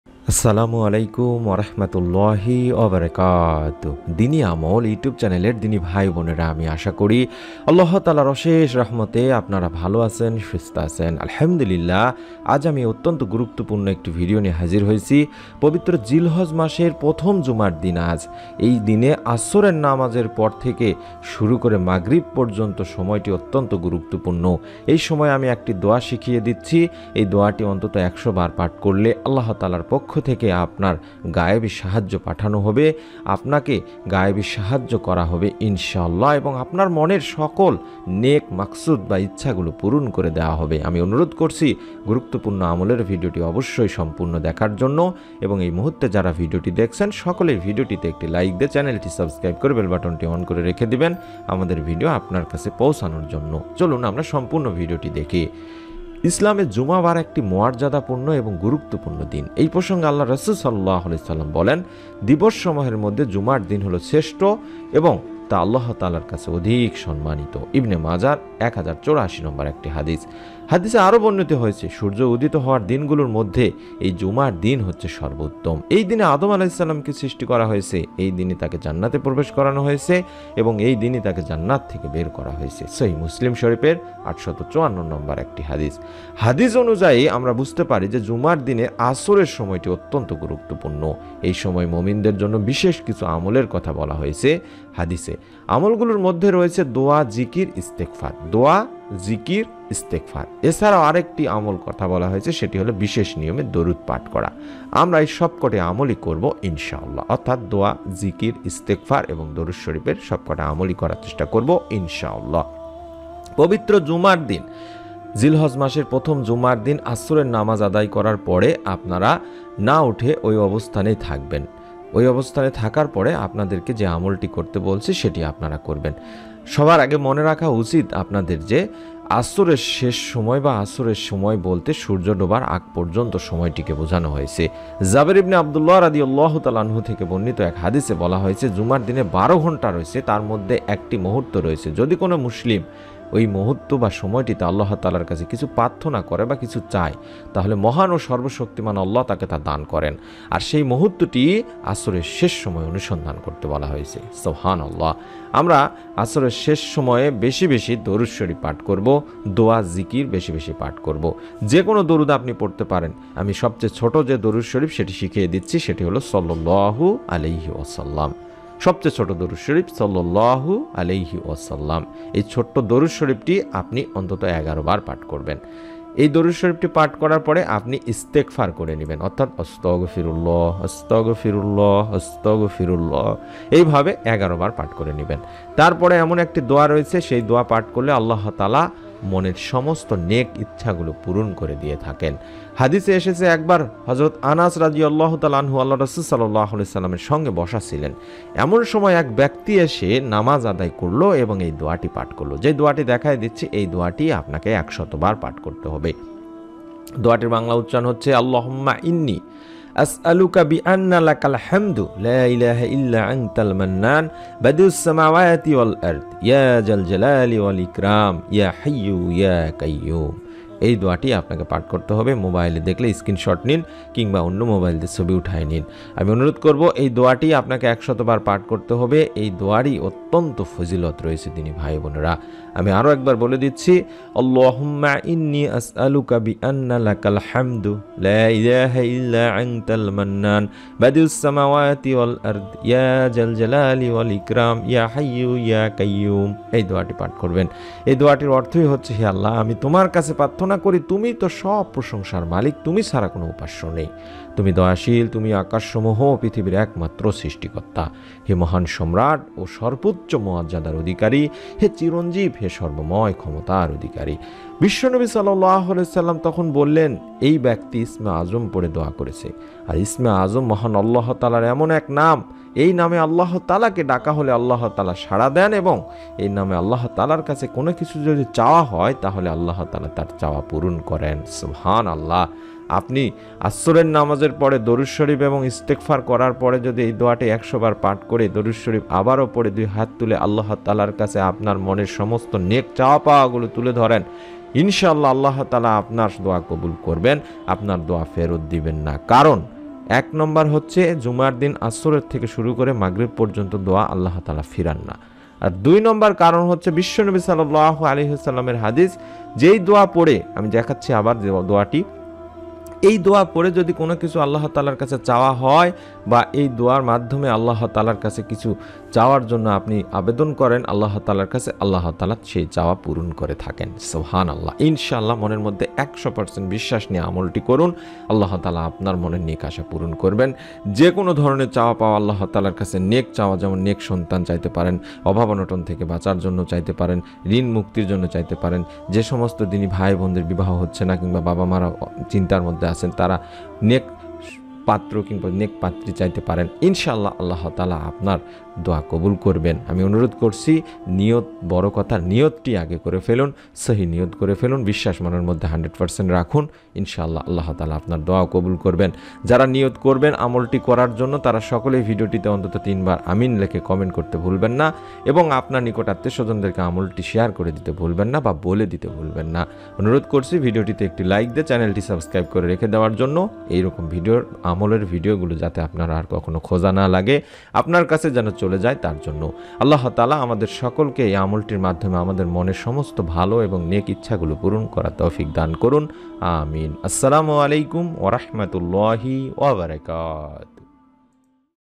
জিলহজ মাসের প্রথম জুমার দিন আসরের নামাজের পর দোয়াটি ১০০ বার পড়ুন । মনের সকল আশা পূরণ হবে Dini Amol
জিলহজ মাসের প্রথম জুমার দিন আসরের নামাজের পর দোয়াটি ১০০ বার পড়ুন । ফেরেশতা নেমে আসবে গায়েবী সাহায্য করার জন্য । মনের সকল আশা পূরণ হবে

জুমার দিন আসরের পরের আমলঃ

ইসলামে জুমাবার একটি মর্যাদাপূর্ণ দিন। এ প্রসঙ্গে রাসুলুল্লাহ (স.) বলেন, ‘দিবসসমূহের মধ্যে জুমার দিন শ্রেষ্ঠ এবং তা আল্লাহর কাছে অধিক সম্মানিত।’ (ইবনে মাজাহ: ১০৮৪)

হাদিসে আরও বর্ণিত হয়েছে, ‘সূর্য উদিত হওয়ার দিনগুলোর মধ্যে জুমার দিন সর্বোত্তম। এই দিনে আদম (আ.)-কে সৃষ্টি করা হয়েছে। এই দিনে তাঁকে জান্নাতে প্রবেশ করানো হয়েছে এবং এই দিনে তাঁকে জান্নাত থেকে বের করা হয়েছে।’ (মুসলিম: ৮৫৪)

হাদিস অনুযায়ী জুমার দিনের আসর সময়টি খুবই গুরুত্বপূর্ণ। এ সময় মুমিনদের জন্য বিশেষ কিছু আমলের কথা বলা আছে হাদিসে। আমলগুলোর মধ্যে রয়েছে দোয়া, জিকির, ইস্তেগফার ইত্যাদি। এছাড়াও আরেকটি আমলের কথা বর্ণিত হয়েছে। সেটি হলো- বিশেষ নিয়মে দরুদ পাঠ।

নবীজির ওপর দরুদ পাঠ মহান ইবাদতঃ

হাদিসে কুদসিতে এসেছে, ‘তোমরা জুমার দিনে বেশি বেশি দরুদ পড়ো। কারণ, জিব্রাইল (আ.) এইমাত্র আল্লাহ তায়ালার বাণী নিয়ে হাজির হলেন। আল্লাহ তাআলা বলেছেন, ‘পৃথিবীতে যখন কোনো মুসলমান আপনার ওপর একবার দরুদ পড়ে, আমি তার ওপর দশবার রহমত নাজিল করি এবং আমার সব ফেরেশতা তার জন্য দশবার ইস্তেগফার করে।’ (তারগিব: ৩/২৯৯)

দুরুদ শরীফ বাংলা উচ্চারণঃ

-আল্লাহুম্মা সল্লি আ’লা মুহা’ম্মাদিওঁ-ওয়া আ’লা আলি মু’হাম্মাদ।

মনের সব ইচ্ছে পূরণ হয় যে আমলেঃ

উচ্চারণ-  ‘আল্লাহুম্মা ইন্নি আসআলুকা বিআন্না লাকাল হামদু, লা-ইলাহা ইল্লা আংতাল মান্নান, বাদিউস সামাওয়াতি ওয়াল আরদ্, ইয়া জালজালালি ওয়াল ইকরাম, ইয়া হাইয়ু ইয়া ক্বাইয়্যুম।’

অর্থ- ‘হে আল্লাহ! আমি তোমার কাছে প্রার্থনা করি। তুমিই তো সব প্রশংসার মালিক, তুমি ছাড়া কোনো উপাস্য নেই। তুমি দয়াশীল। তুমিই আকাশসমূহ ও পৃথিবীর একমাত্র সৃষ্টিকর্তা! হে মহান সম্রাট ও সর্বোচ্চ মর্যাদার অধিকারী, হে চিরঞ্জীব, হে সর্বময় ক্ষমতার অধিকারী।’

মহান আল্লাহ আমাদের সকলকে এই আমলটি করার তাওফিক দিন । আমিন ।